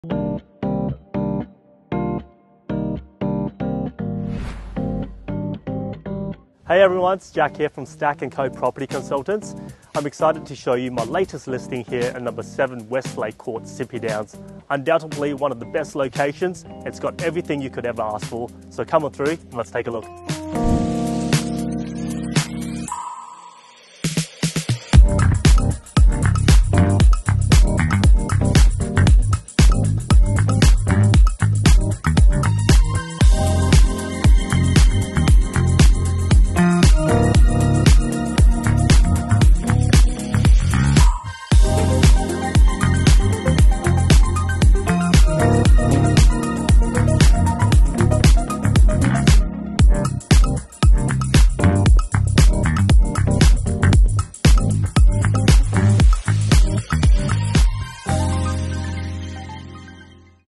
7 Westlake Court, Sippy Downs
Jack Ryan and the team at Stack & Co are proud to present a truly rare opportunity for one lucky family that has been waiting for that perfect home to come on the market.

This large family home is located in one of the most desirable cul-de-sac streets in Sippy Downs voted by the local residents in the area for its quiet and family-friendly location, but also close enough to walk to the local shopping complex and schools.

LOCATION and SIZE are two words that come to mind when describing this home. Boasting multiple large living spaces, four bedrooms + a large study area that can be turned into a fifth bedroom, the perfect layout for growing families.

The East facing family home is on a generous 771 sqm flat block. With plenty of room for entertaining your guests and loved ones around your large inground saltwater pool and a large undercover alfresco area.

The large drive-through shed is perfect for the tinkerers of the family, has room for a workshop and also plenty of room to park your prized possession, whether it's a unique car/bike or jet skis.

Impeccably maintained and cared for by owners, and wonderfully located Close to schools, public transport, shopping and motorway access. Your new home is move-in ready today.

Features you'll love:

• Freshly painted
•  Large Renovated Kitchen
•  Large 4.8 x 6 Shed
•  Study Room can be converted to extra bedroom
•  Quiet Cul-de-sac Street
•  Spacious natural light-filled home
•  Multiple grand living areas
•  Ensuite with Walk-in Robe in the Main bedroom
•  In-ground backyard Saltwater Pool
•  Large Undercover outdoor entertainment area
•  Abundance of parking/ caravan/ boat
•  Ample room for playground/ trampoline/shed
•  Close to schools, public transport, shopping and motorway access
• 6.3 Kw Solar system installed
•  Pull Down stairs in the garage for roof storage

This property is absolutely not one to be missed for anyone looking to create their dream home with a grand proportion in mind. You and your family will love living at 7 Westlake Court Sippy Downs.

Everything about this home excites us, and we can't wait to show you through it! If you require further information, please get in touch with Jack on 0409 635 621 today.